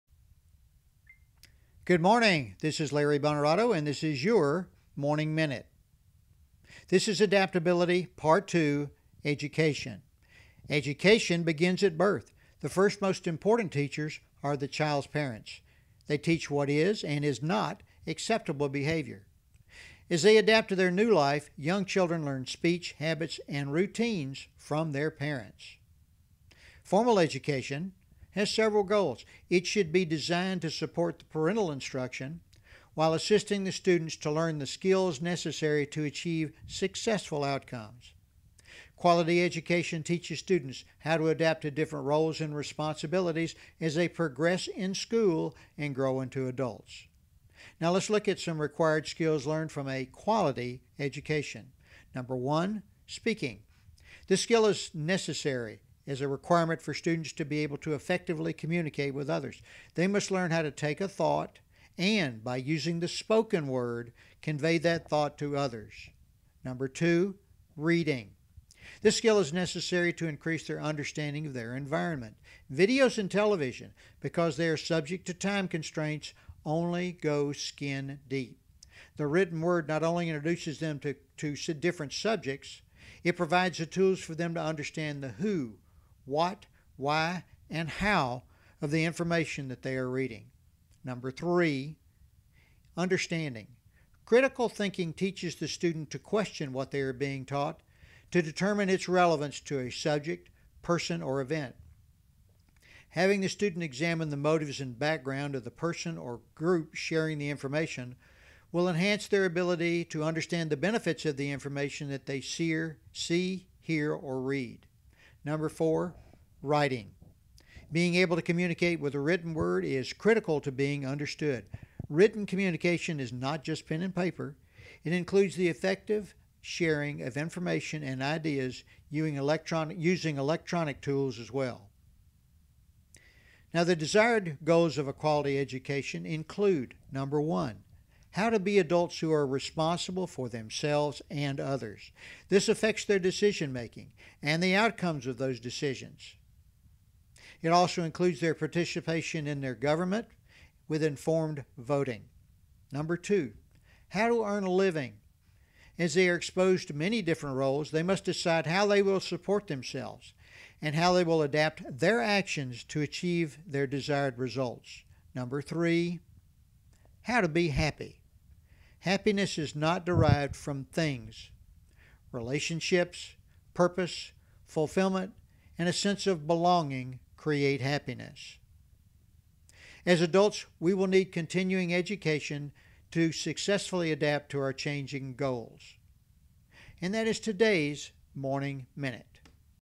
Morning Minute 10/5/22 Adaptability in Education
This short message shares the ways to use in creating and/or expanding your organizational footprint while increasing revenues.